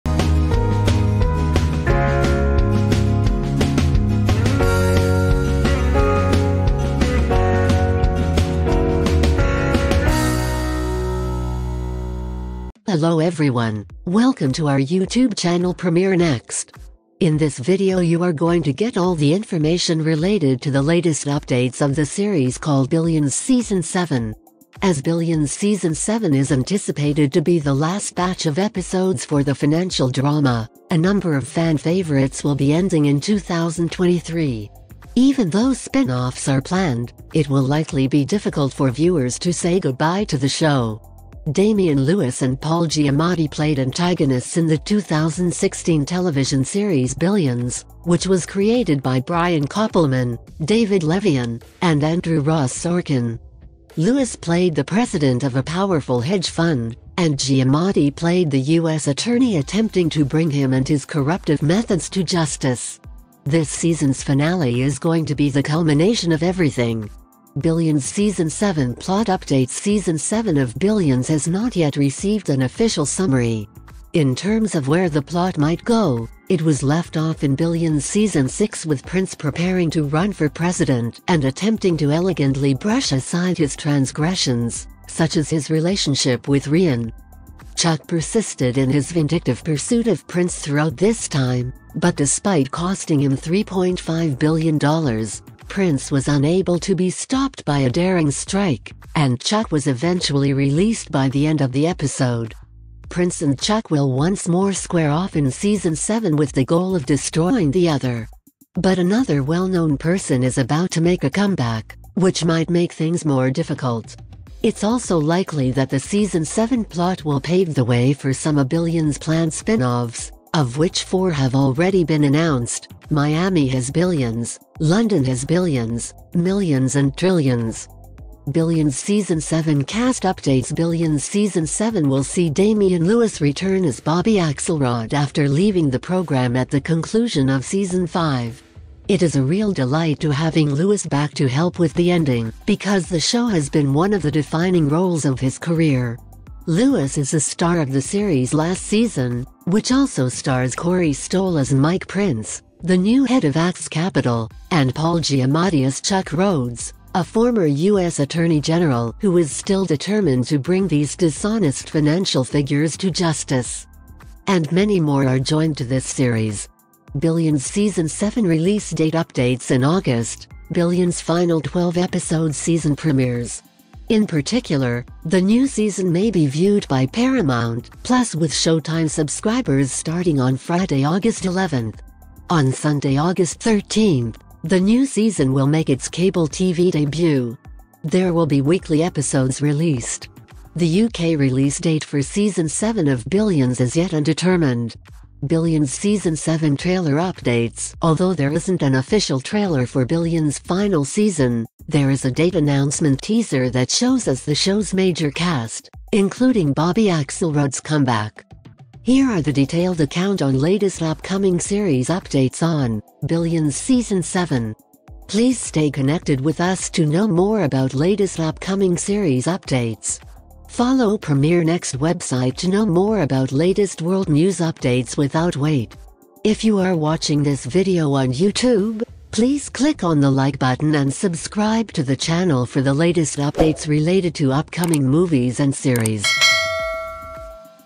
Billions Season 7: Official Trailer Breakdown - Premiere Next
Billions Season 7: Official Trailer Breakdown
in this video, you will get the info related to Billions Season 7 release date, cast, and key details.

will damien lewis be back on billions season 7"
billions season 7 cast imdb"

Technology updates
Netflix Series
Netflix Movies
Premiere Next